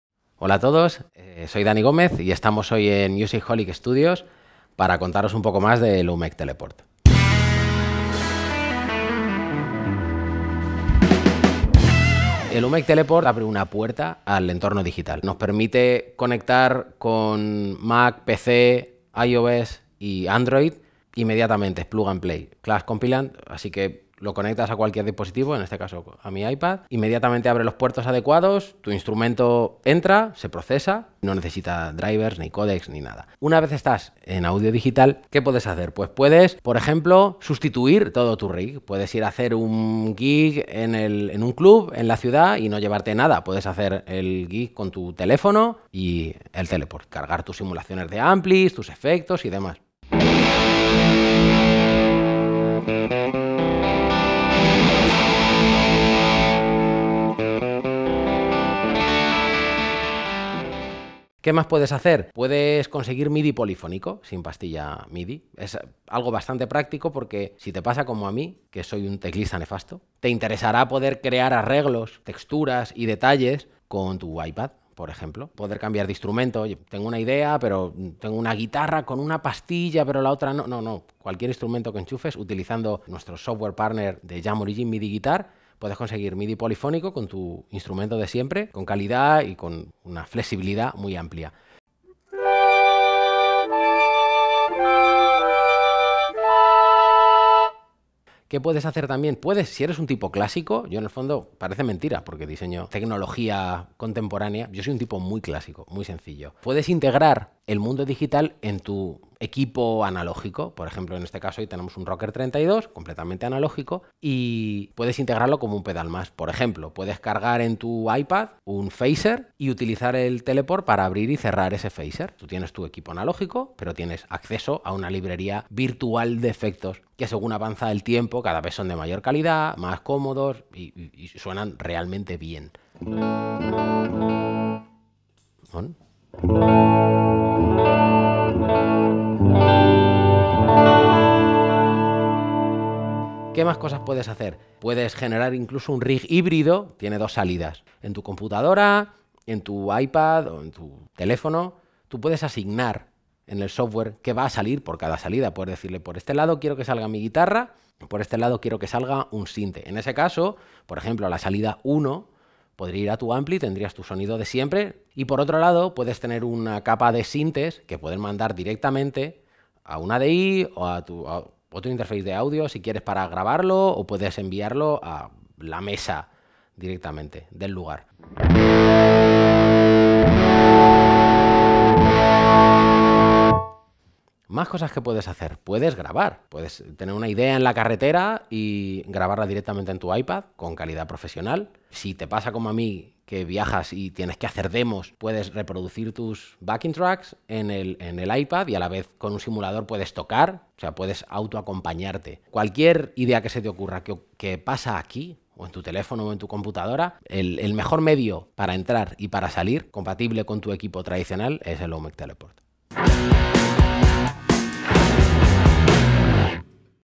[EXCLUSIVO 1/6] Danny Gómez te presenta el Audio Interface OMEC Teleport de Orange Amplifiers
Detrás del nuevo Orange OMEC Teleport tenemos al guitarrista Danny Gómez (La Quinta Estación, Nacho Cano, Gerónimo Rauch, “We Will Rock You"), que nos cuenta como surgió la idea de crear este pedal audio Interface: "Como músico siempre en gira, necesitaba algo sencillo que me permitiera usar el software y aplicaciones que empleo habitualmente en el estudio".